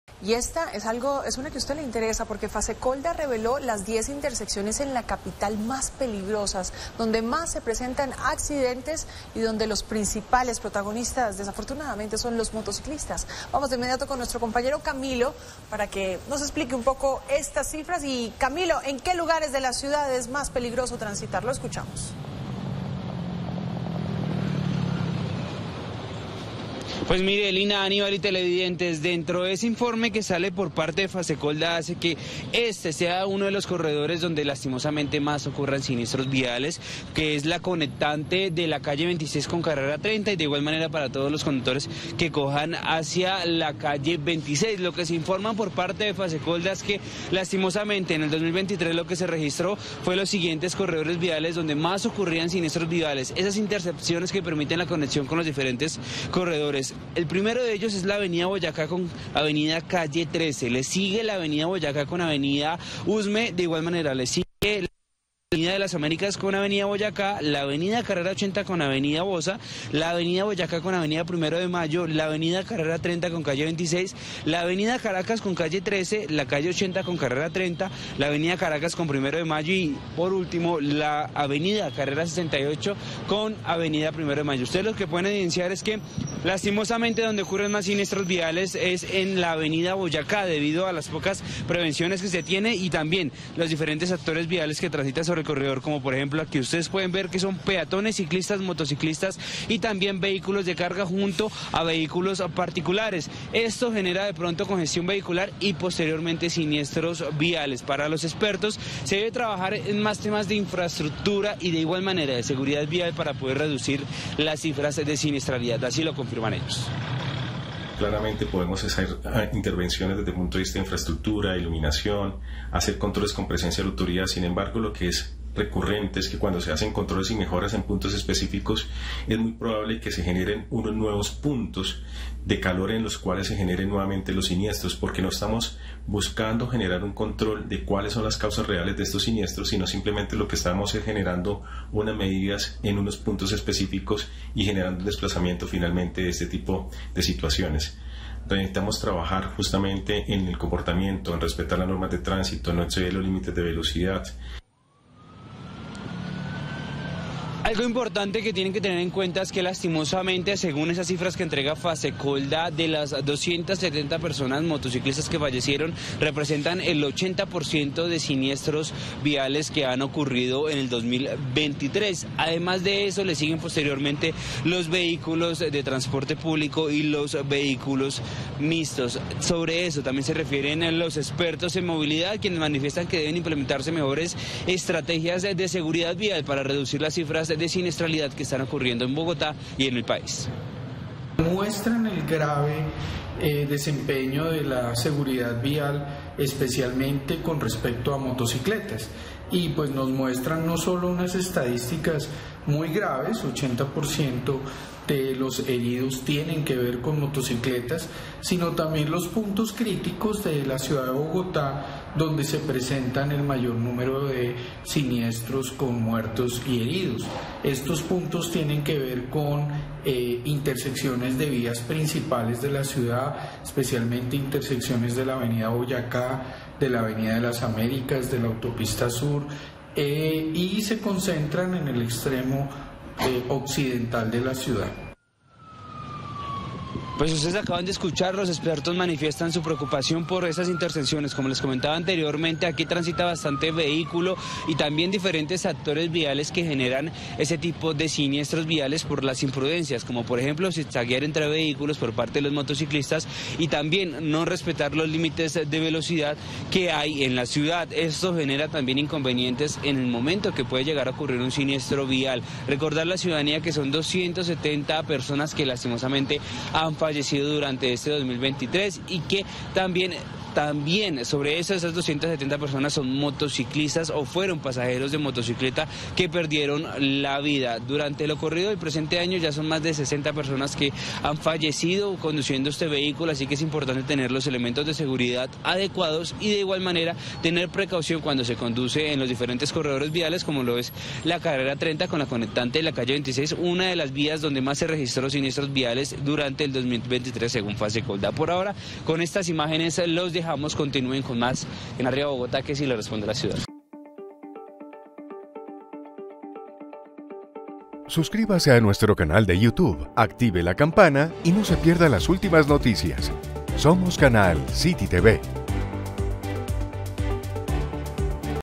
Fasecolda reveló las 10 intersecciones más peligrosas de Bogotá | CityTv
Fasecolda, la Federación de Aseguradores Colombianos, ha entregado detalles sobre las 10 intersecciones en las que más accidentes de tránsito se registran en Bogotá. Este informe cobra relevancia en un contexto donde la seguridad vial es una preocupación constante, especialmente tras los alarmantes datos de mortalidad en accidentes viales en la capital.